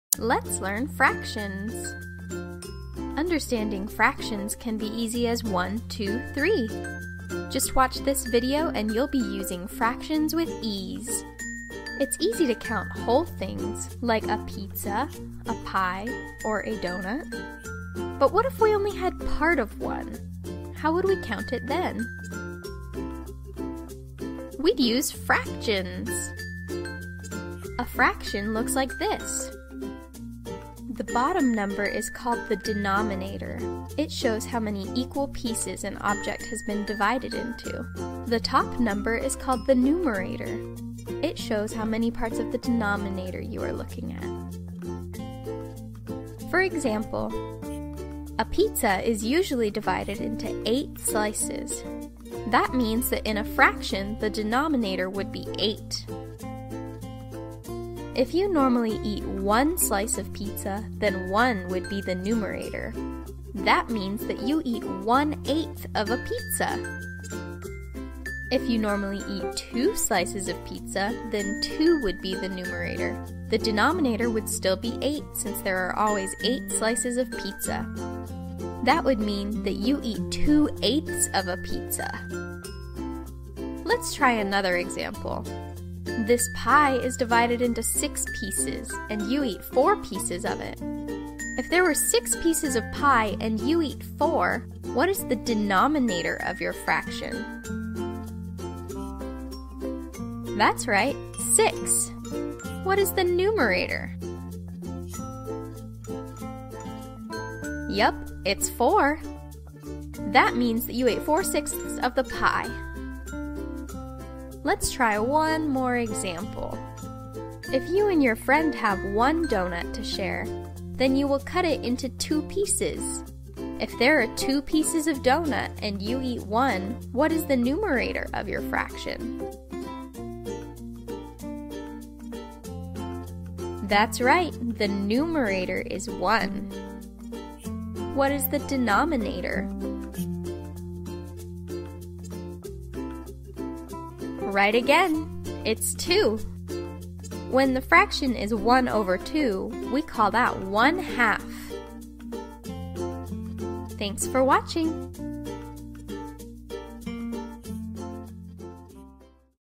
Let's Learn Fractions! | Understanding Math for Children | Kids Learning Videos
Learning Fractions

This video teaches the mathematical principle of fractions in an easy to understand way for children and kids to learn.  This video uses visual representations to point out common fractions that we see in everyday life.   This will help children to learn and apply fractions each day.